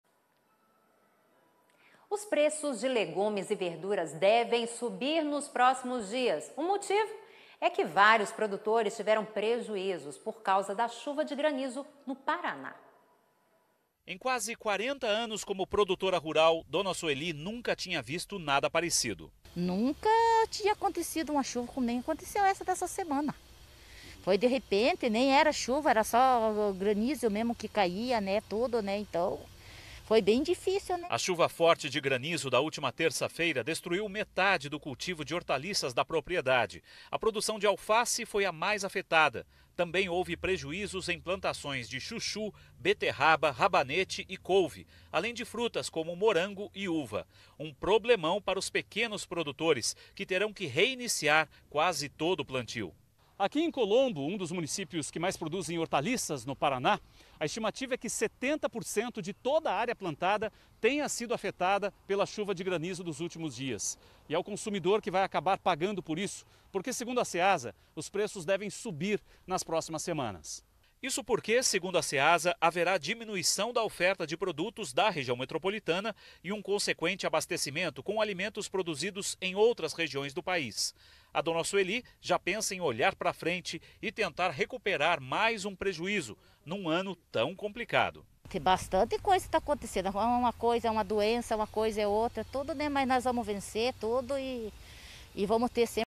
Chuva de granizo gera prejuízos a produtores de hortaliças | AgroMais
Vários produtores de legumes e verduras tiveram prejuízos por conta da chuva de granizo que atingiu algumas cidades do Paraná na terça-feira.